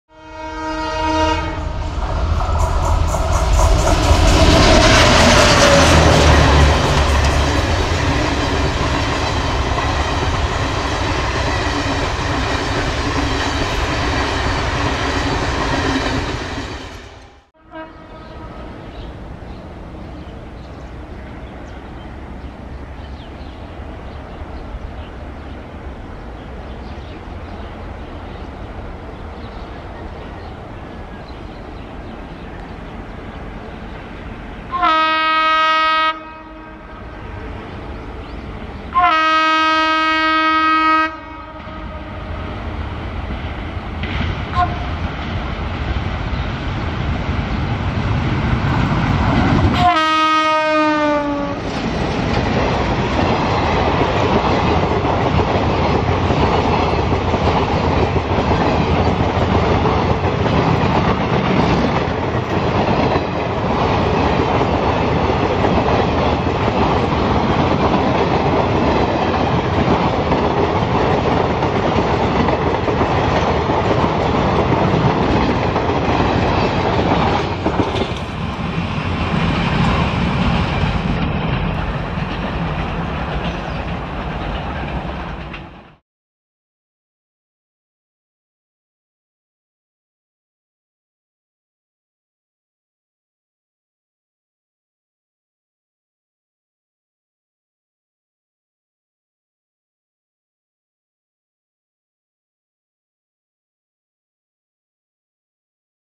Twin ALCo WDM3A Rockstars hauling the 15929 Tambaram - Dibrugarh Express through Tetelia
15929 Tambaram – Dibrugarh Express (weekly) hauled by ALCo WDM3A twins: 16106R (leading) and 16178R (trailing) of DLS Andal (UDL), ER; captured at Tetelia, Assam.